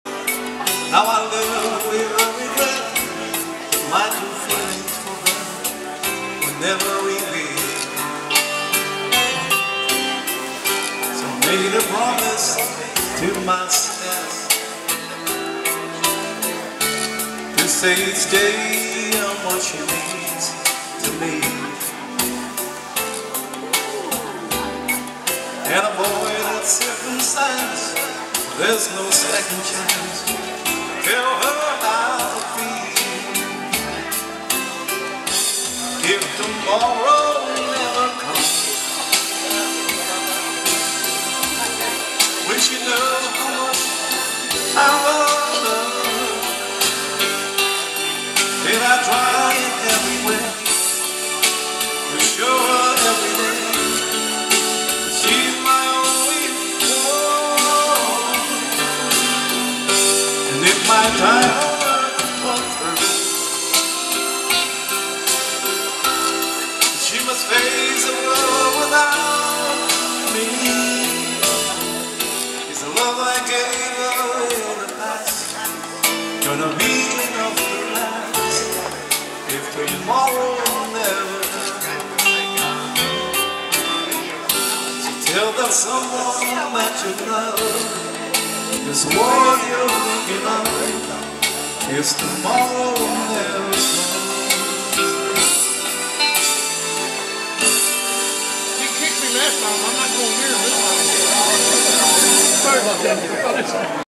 Bolero - Lance & Elena Sexton
Lance & Elena Sexton dancing Bolero at the USA Dance Space Coast chapter's dance on October 30, 2016.